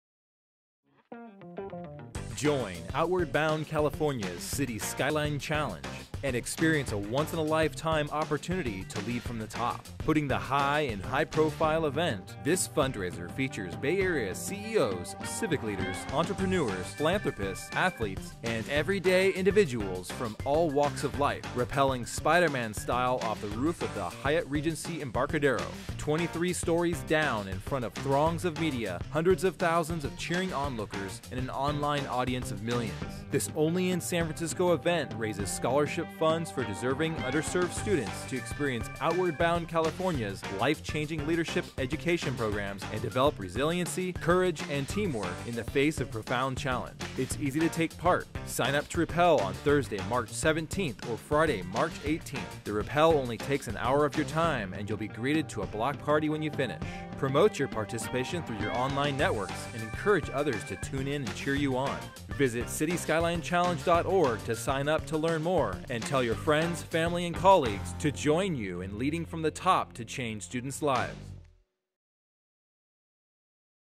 City Skyline Challenge - Rappel with Us!
Join the fun and get the thrill of a lifetime all while raising crucial funds to get kids outside and learning life skills and leadership.